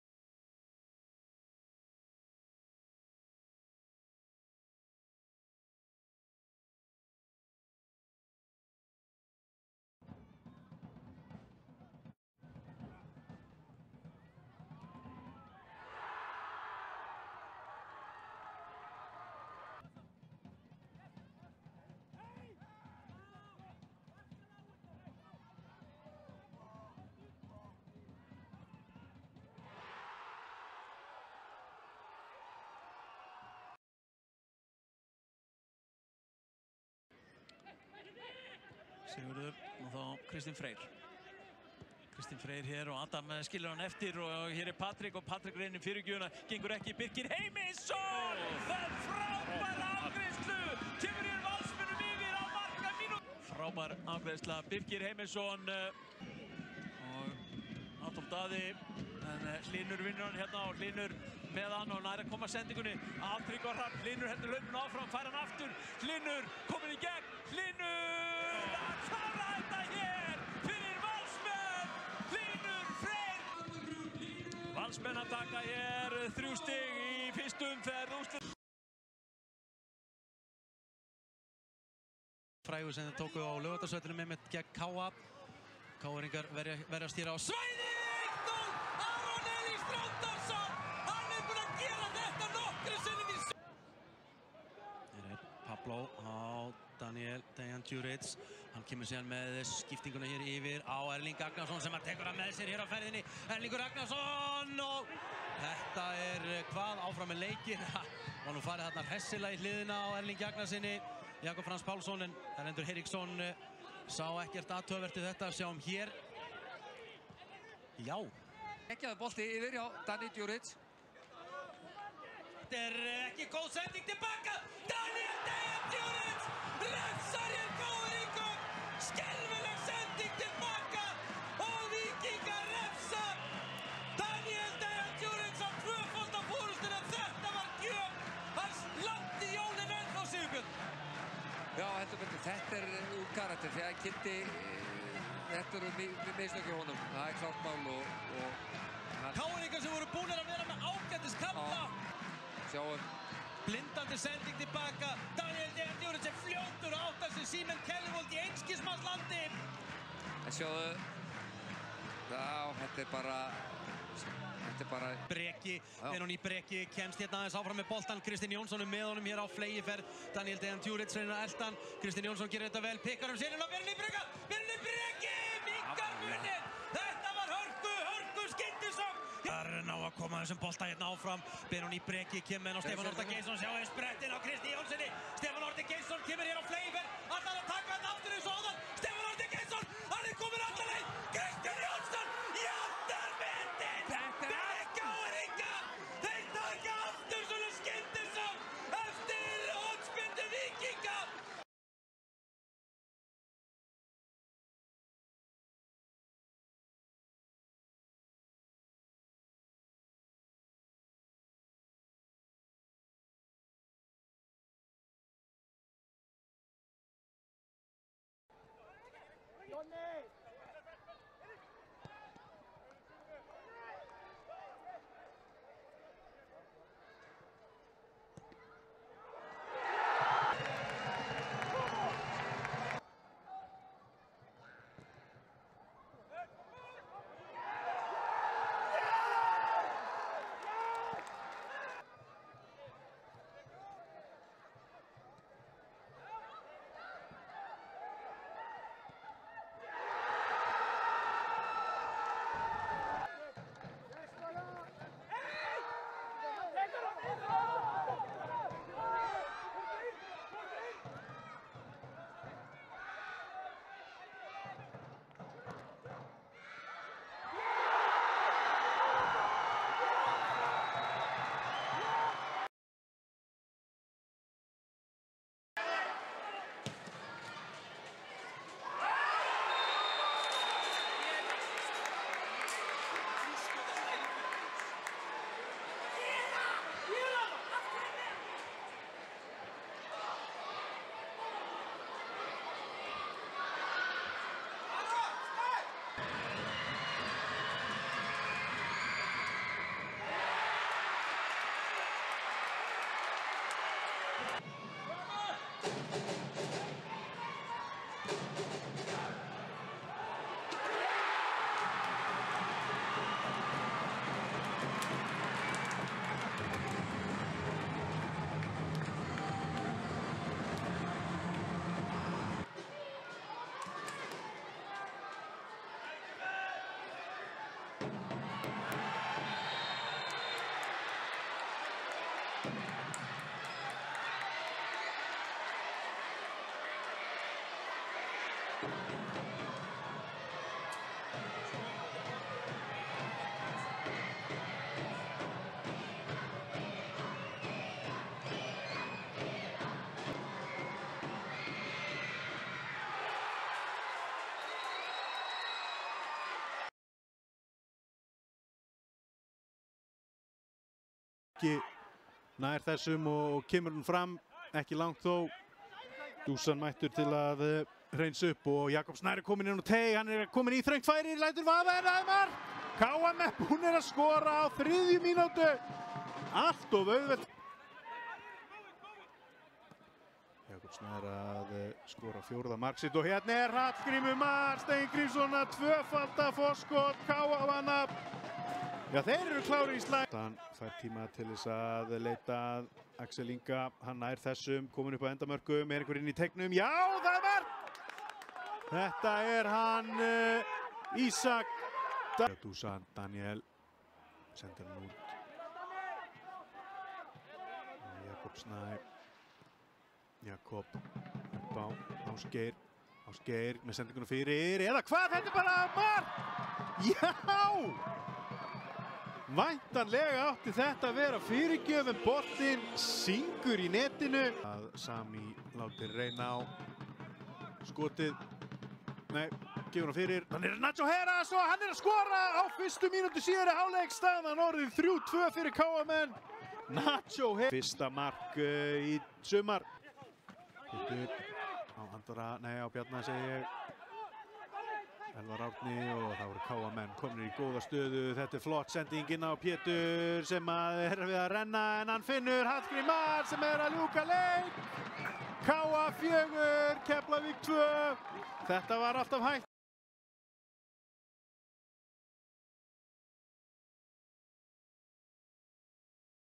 Besta Deildin 2023 (Championship/relegation round - day 1, men)
Here you can see all goals from Icelandic league.
Championship/relegation round - day 1
6 games = 20 goals

Results:
CHAMPIONSHIP ROUND:
Breidablík - FH Hafanarfjordur 0:2
Valur - Stjarnan 2:0
Víkingur - KR Reykjavík 2:2

RELEGATION ROUND:
Fylkir - ÍBV 2:2
HK Kopavogur - Fram 1:1
KA Akureyri - Keflavík 4:2